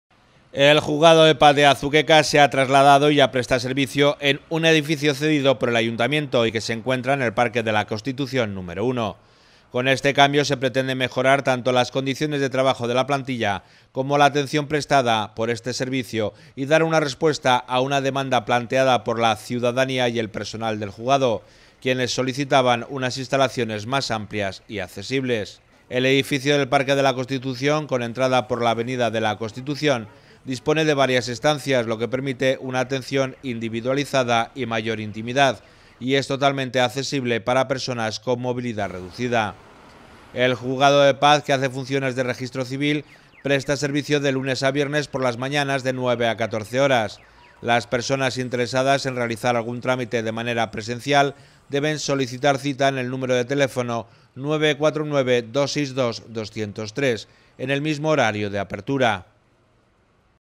El Juzgado de Paz de Azuqueca ya funciona en su nueva ubicación
El Juzgado de Paz de Azuqueca de Henares se ha trasladado y ya presta servicio en un edificio cedido por el Ayuntamiento y que se encuentra en el parque de La Constitución, número 1.